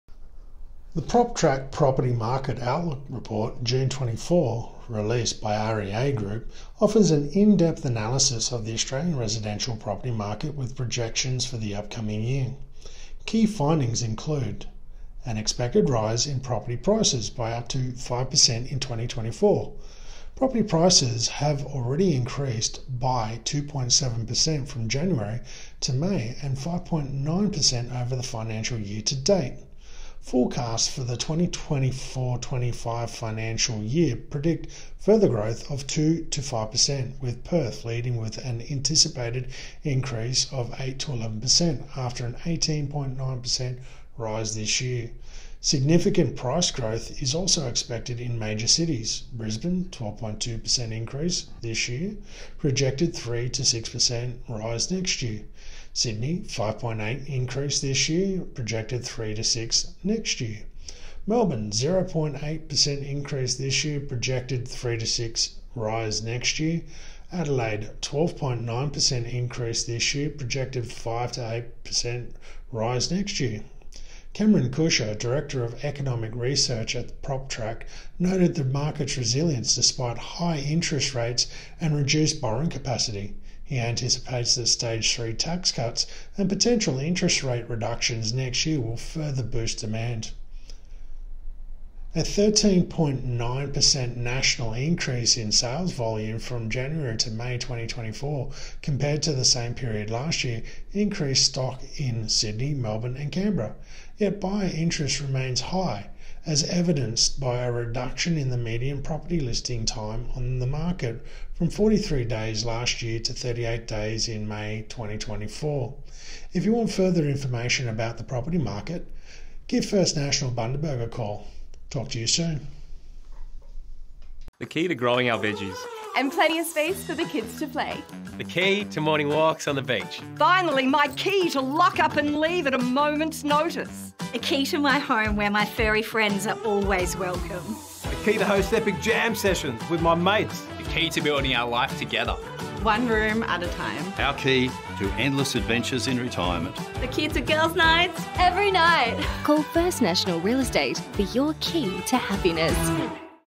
Australian Property Prices on the Rise: Expect a 2% to 5% increase in the 2024-25 financial year.
we dive into the PropTrack Property Market Outlook Report June 2024! 📊 This comprehensive analysis reveals key insights and projections for the Australian residential property market in the coming year.

🏠 Key Highlights:

Property Prices on the Rise: Expect a 2% to 5% increase in the 2024-25 financial year.

City-Specific Growth: Perth leads with an anticipated 8% to 11% rise, while Brisbane, Sydney, Melbourne, and Adelaide also show significant upward trends.

National Sales Surge: A 13.9% increase in sales volumes from January to May 2024.

 Market Resilience: Despite high-interest rates, buyer demand remains robust.

📈 Insights from Cameron Kusher, Director of Economic Research at PropTrack:

Resilience of the property market.
Impact of potential interest rate reductions and tax cuts on buyer demand.
Decrease in median listing time, signaling high buyer interest.